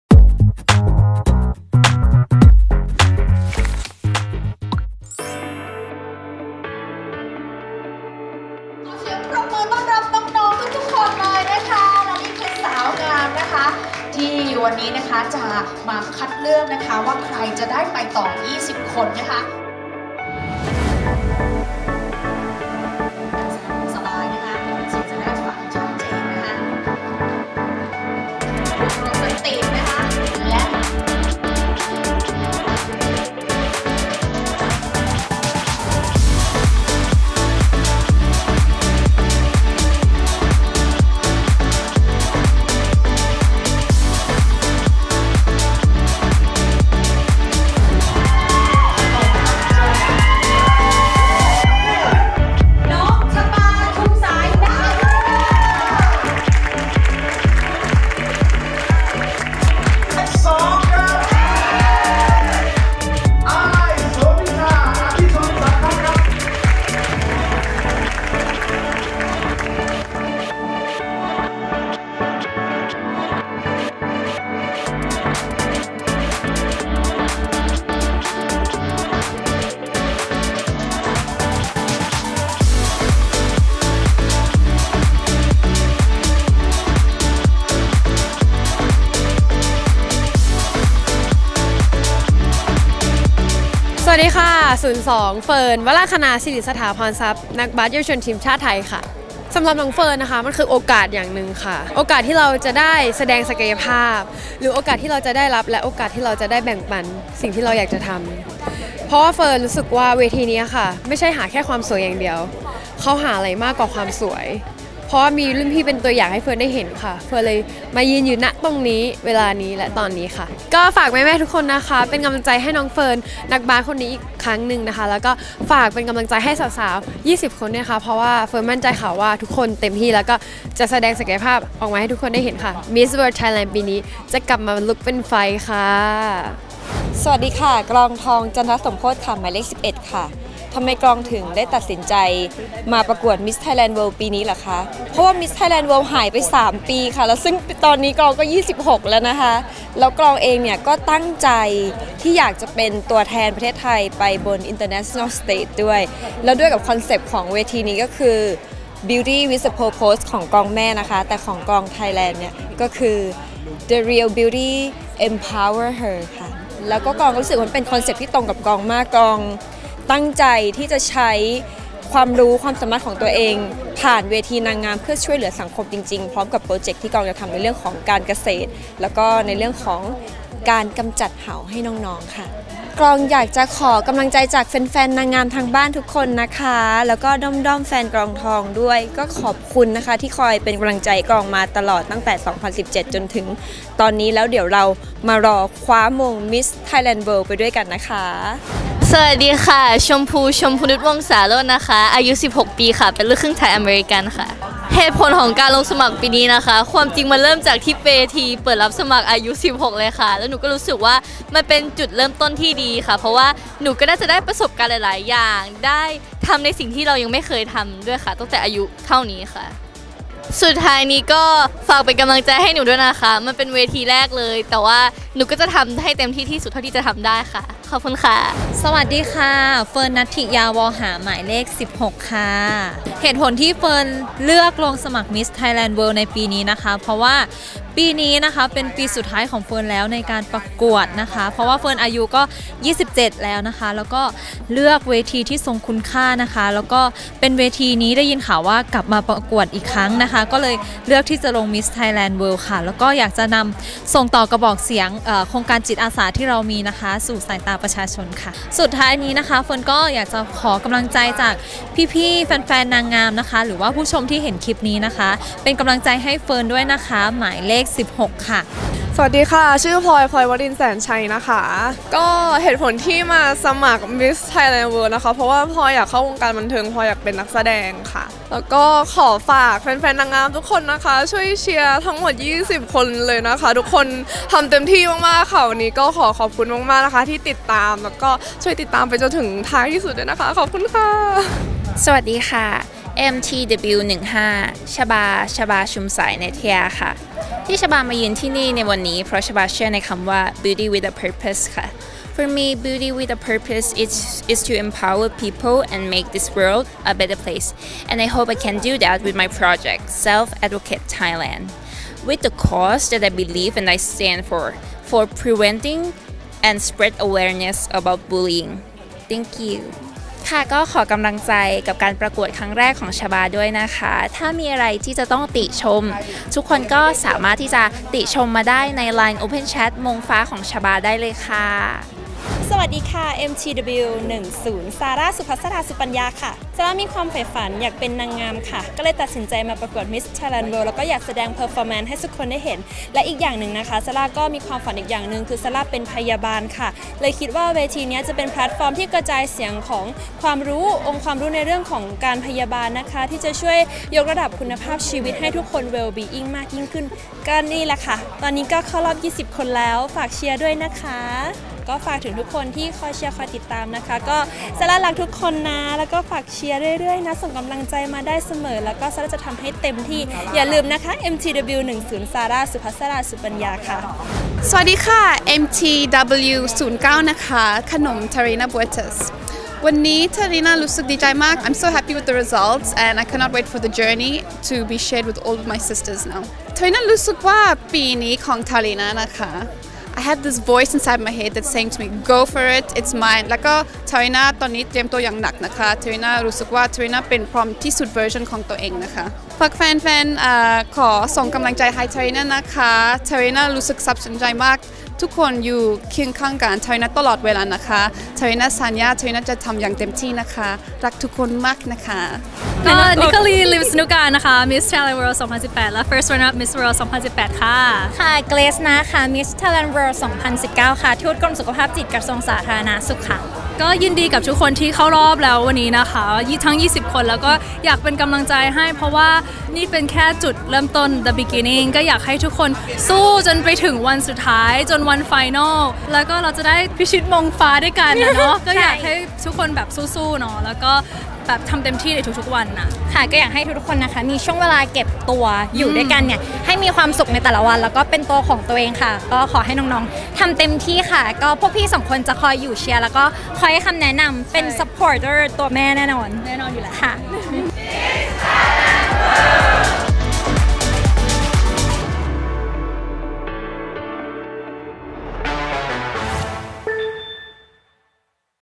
รอบคัดเลือก 20 คนสุดท้าย Miss Thailand World 2023 | หลงมา Talk! Special
บรรยากาศรอบคัดเลือก 20 คน Miss Thailand World 2023 คัดเลือกโดยคณะกรรมการเพื่อค้นหาสาวงามตัวแทนประเทศไทยในปีนี้ เข้าร่วมการประกวด Miss World 2023 เพื่อชิงมงฟ้า ณ ประเทศอินเดีย​